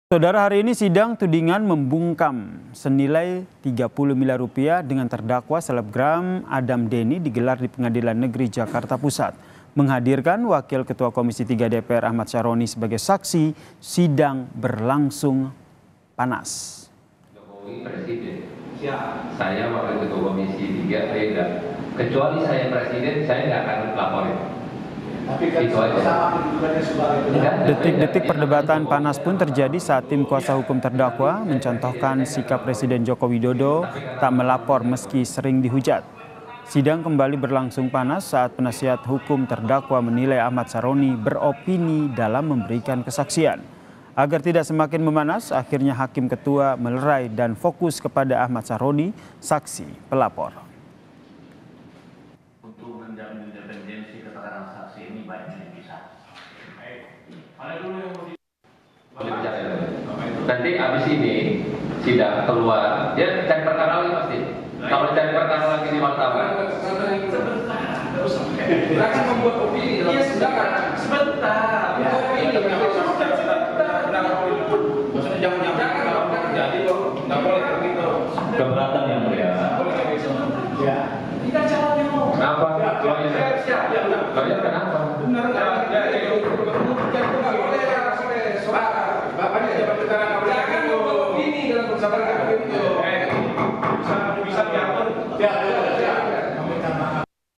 Momen Debat Ahmad Sahroni di Sidang Selebgram Adam Deni
KOMPAS.TV - Panas! Ini Momen Debat Ahmad Sahroni di Sidang Selebgram Adam Deni